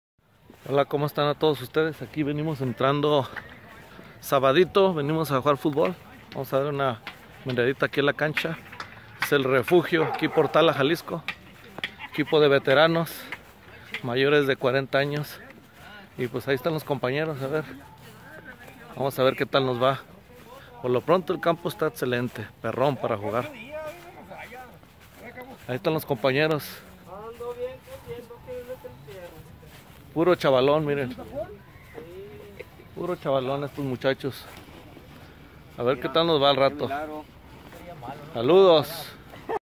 Reconocimiento de la cancha en Tala Jalisco
No hay nada mas chingon que pisar una cancha de futbol. Nos metimos a la cancha de Tala y les ganamos a Los de la Prepa 3 a 2 a puro riñon y con 10 hombres Diciembre 2017